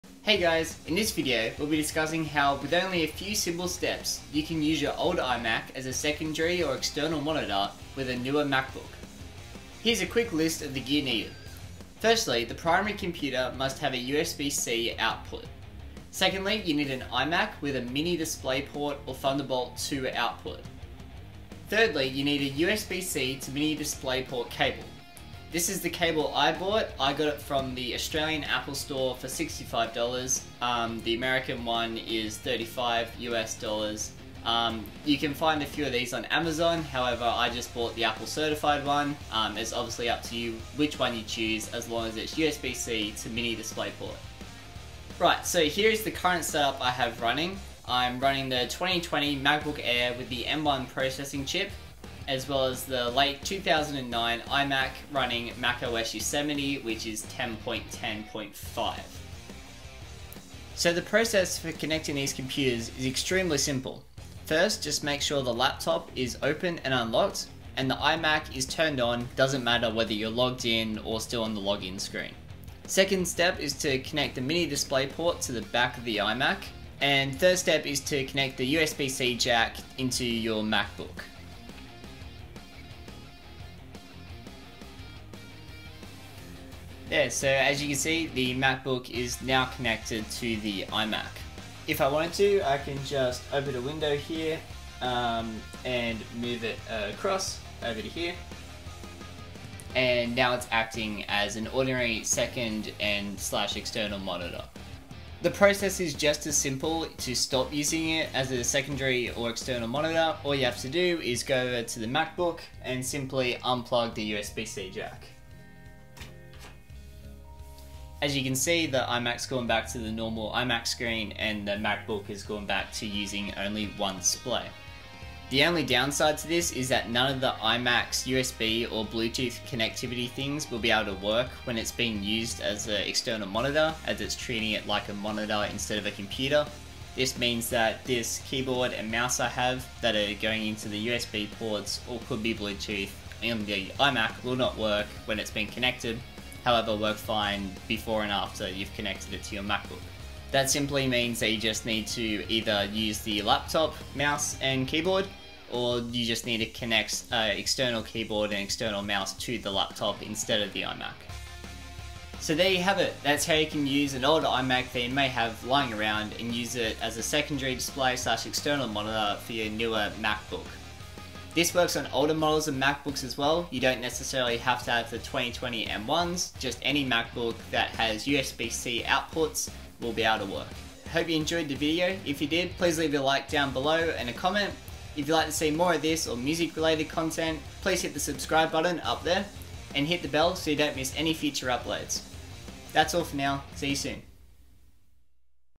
How to use your iMac as an External Display / Second Monitor - with Macbook Air M1
How to Use an Old iMac as a Secondary / External Monitor. This video will show you the step by step process on how to use an old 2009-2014 Apple iMac as an external monitor for newer MacBooks, shown by the 2020 MacBook Air M1. This method utilises the Target Display mode built into these older iMacs, without the complicated Cmd F3 way of doing it.

Do you have an old 27" iMac from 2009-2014 that's getting old but functions just fine? Well that's the exact position I found myself in. In this video you will see how Purchasing a $35.00 Cable from Apple gave my old iMac a new purpose! It's very simple, easy, quick and straight to the point, no fancy tech required!

We will Turn Our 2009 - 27" iMac into an external Display using our 13.3" 2020 M1 Macbook Air! Also works on Apple M1 Mac mini and older MacBook models! No need to invest in a new monitor when you already have one!

Here is the Apple Certified USB-C to MiniDisplay Port Cable used in the video:
(US Store)

Here’s the list of gear needed:
Primary computer with a USB-C output
iMac with a Mini DisplayPort / Thunderbolt 2
USB-C to Mini DisplayPort Cable

Setup used in this video:
2020 Macbook Air with the M1 chip running BigSur 11.1
A late 2009 iMac running MacOS Yosemite (10.10.5)

So the process of connecting these computers is extremely simple:
 First, just make sure your laptop is open and unlocked, and the iMac can either be on the login screen or already  unlocked
Connect the Mini DisplayPort jack to the iMac
Connect the USB-C jack to the MacBook

This video is filmed on an iPhone SE 2nd Gen (product RED) and edited in Final Cut Pro X.

If you would like to see more videos like this or more music related content, hit the subscribe button up there and be sure to hit the bell so you don’t miss any future uploads.